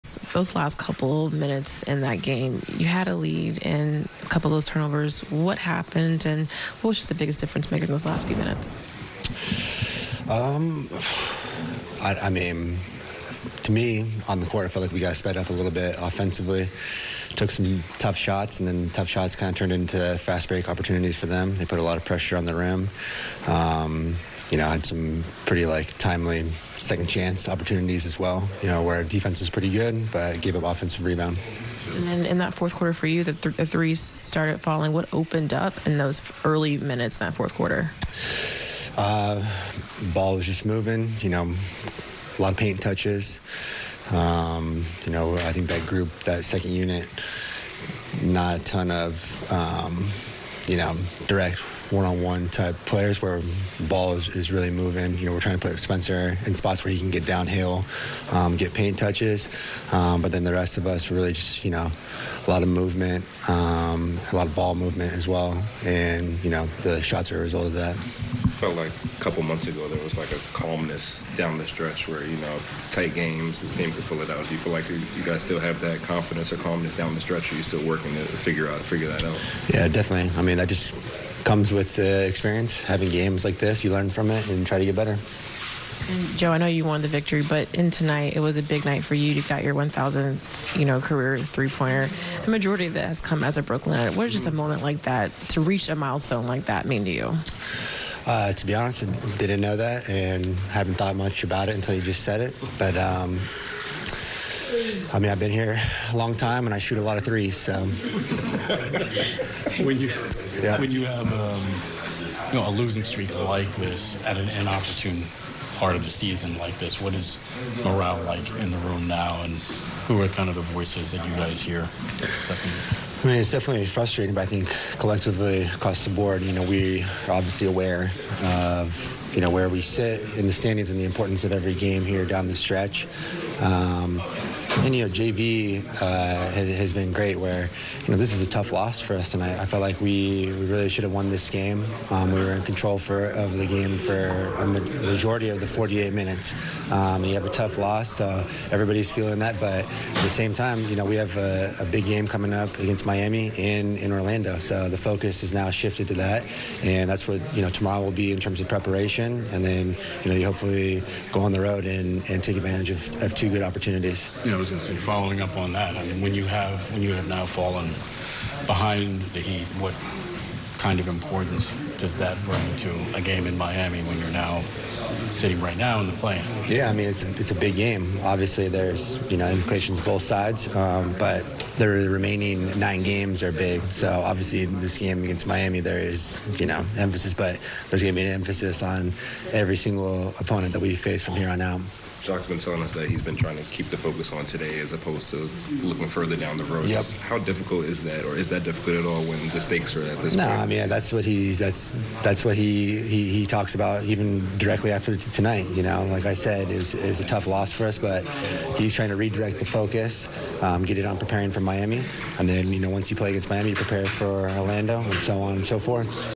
Joe Harris on moving forward amid losing streak
Joe Harris discusses moving forward as season comes to a close, and discusses his 1000th career three pointer milestone.